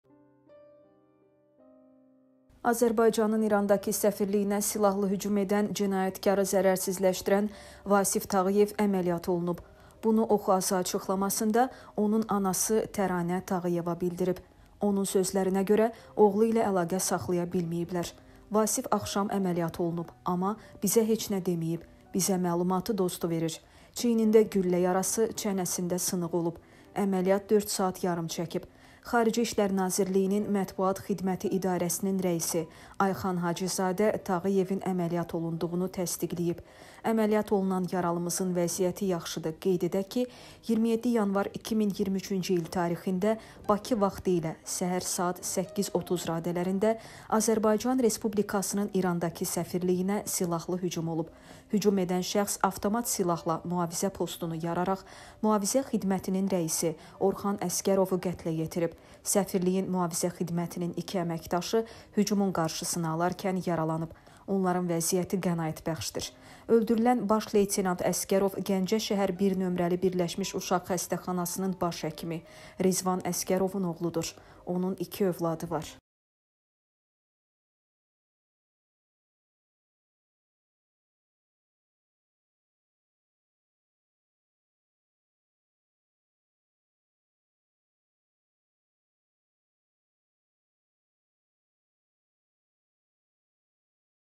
Tehrandakı Səfirliyimizdə terrorçunu zərərsizləşdirən qəhrəman 4 saat əməliyyat olunub
Səfirliyimizə hücum zamanı xəsarət alanların son vəziyyəti açıqlandı
Reklam və Əməkdaşlıq üçün əlaqə nömrəsi:
Azərbaycanın Tehrandakı səfirliyinə hücumun ANBAAN GÖRÜNTÜLƏRİ